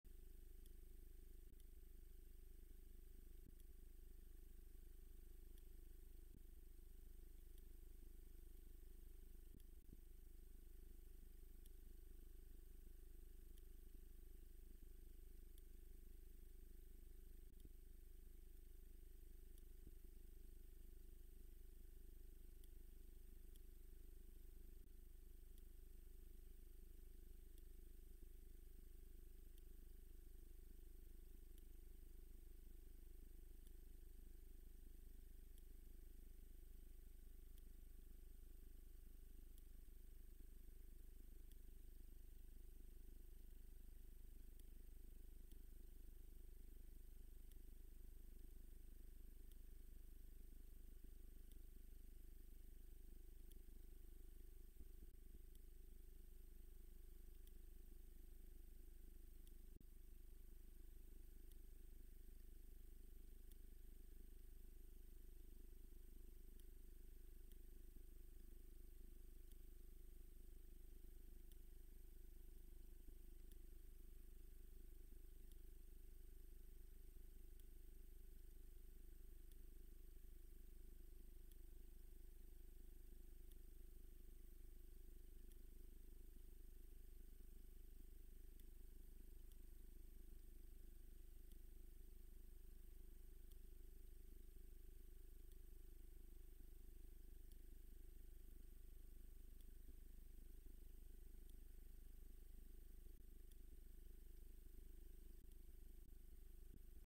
Klekusiowo / Szczytnica ~ deszczowy dzień, zmiana pary w inkubacji jajek 👍💚
Jeśli chcesz dowiedzieć się więcej na temat wydarzeń w gnieździe w Szczytnicy w sezonie 2023, odwiedź